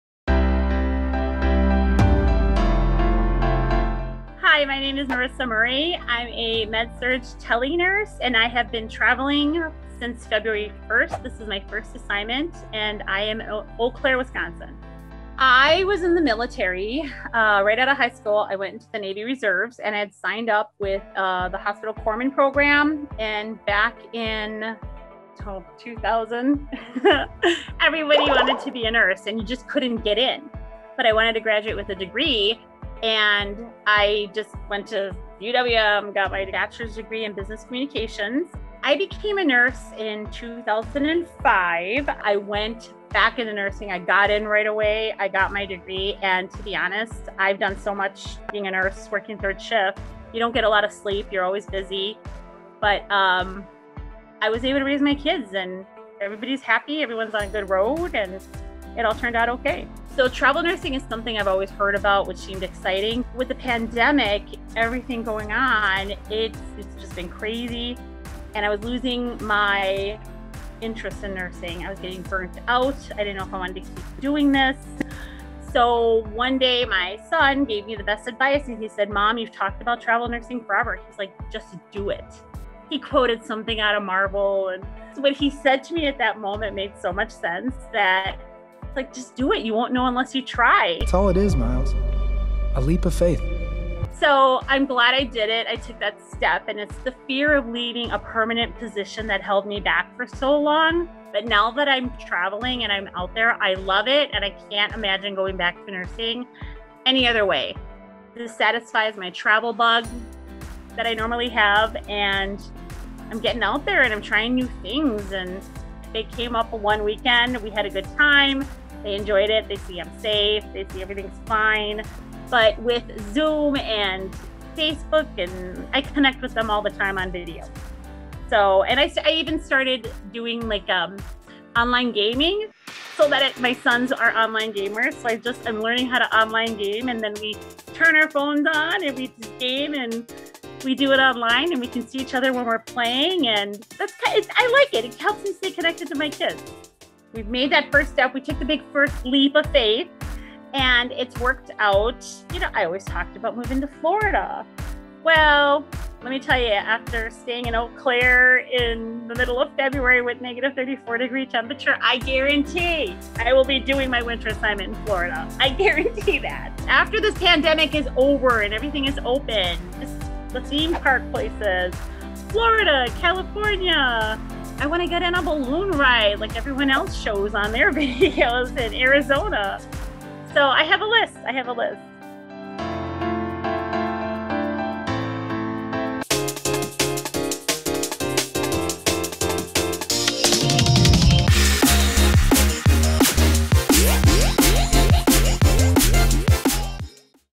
Taking that leap of faith to becoming a travel nurse | What's Your Why - Atlas MedStaff Daily 757
Sometimes it only takes a "leap of faith" to change the direction of your path.

This Med Surg/Tele RN heeded that advice from her son and took the leap into travel nursing and has been able to live out her dream of combining her love of nursing and traveling.

Looking to start your own Atlas Adventure?

Get to know the Atlas MedStaff Team and reach out to this travel nurse's recruiter: